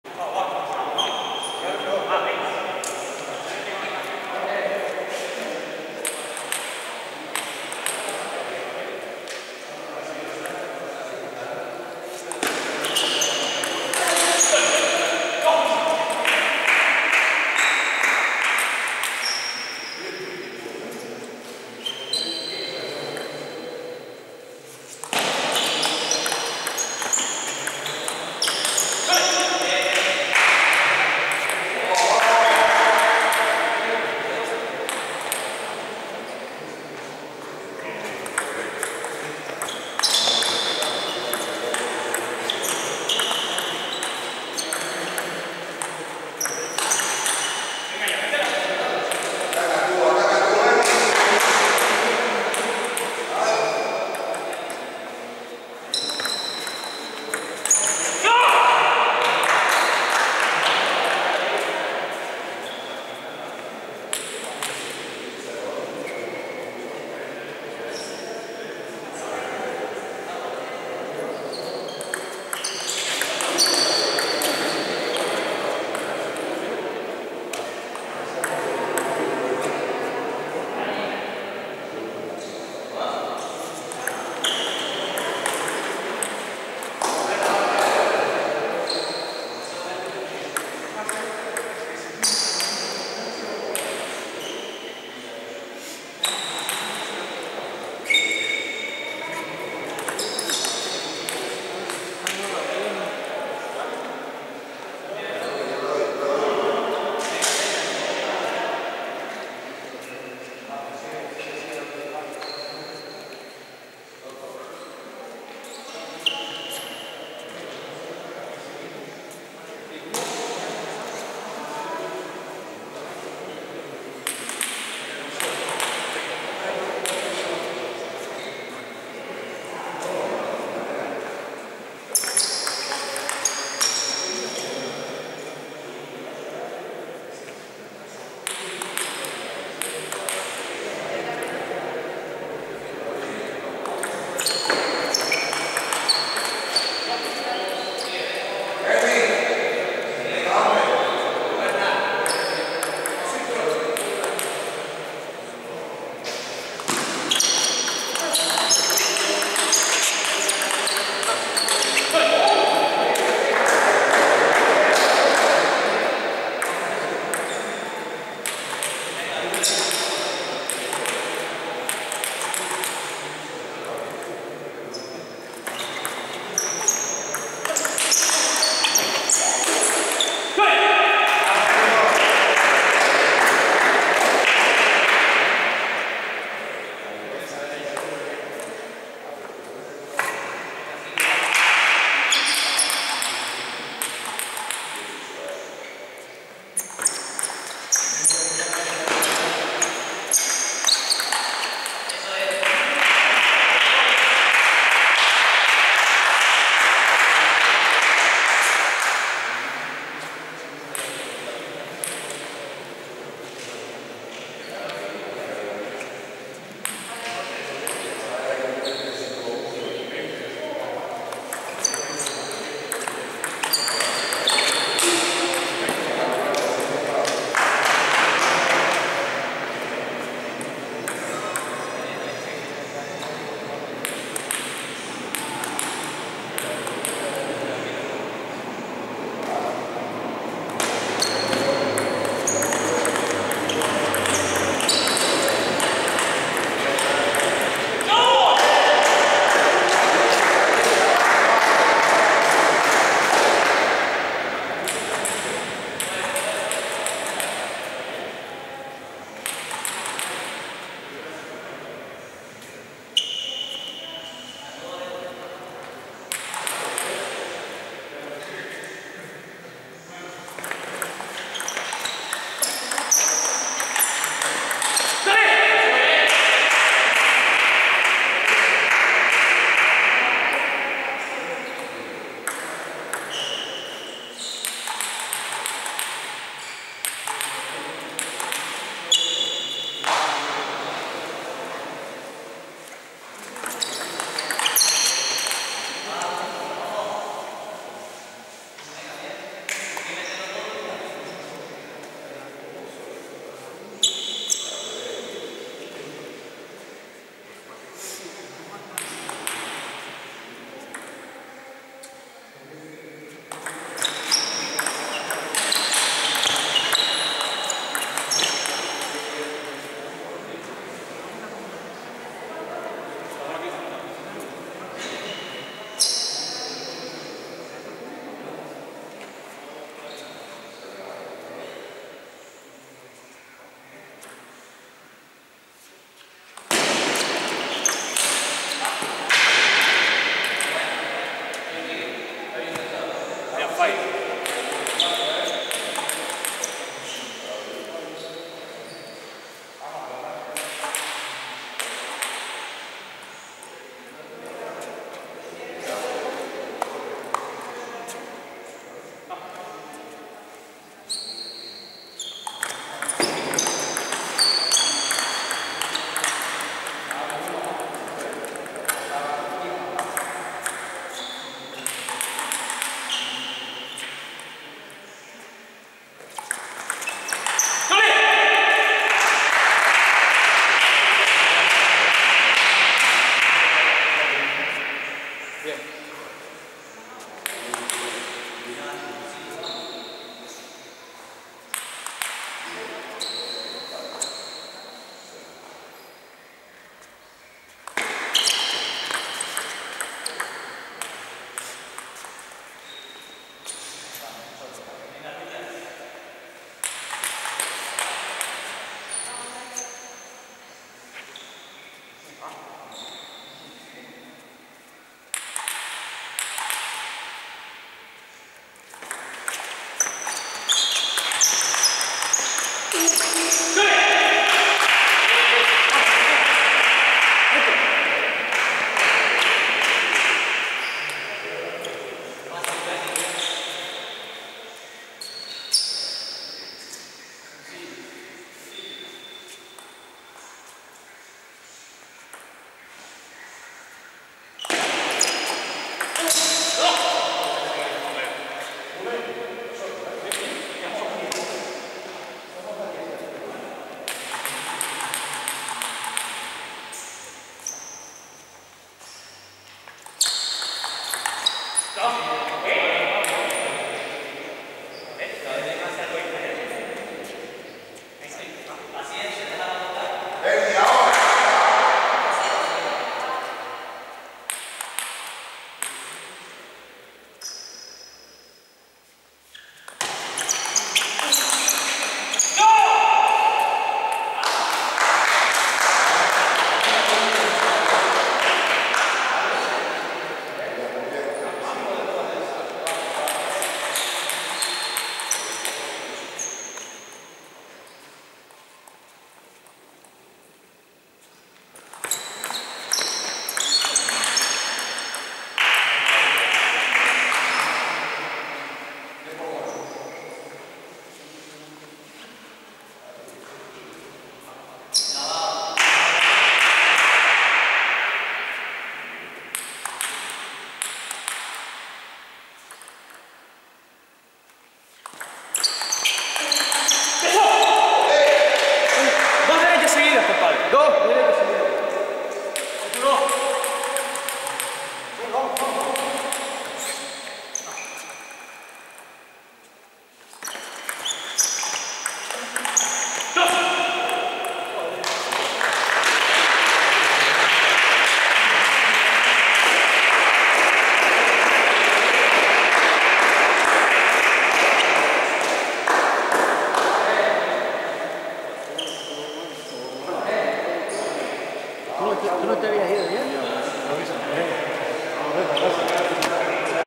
RUBEN MUÑIZ (Tabor) - BERNI ESPINOSA (Circulo Amistad)
Partido SemiFinal del XXIX Torneo Ciudad de La Laguna 2020